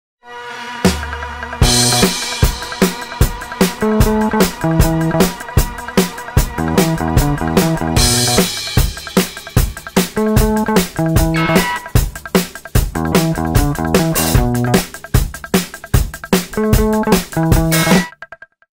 Cocktail COSMOPOLITAN - il tutorial di Petitchef.it
Un cocktail ritornato alla moda negli anni 2000 grazie alla serie televisiva "Sex and the City". Un'ottima idea per una serata tra amiche!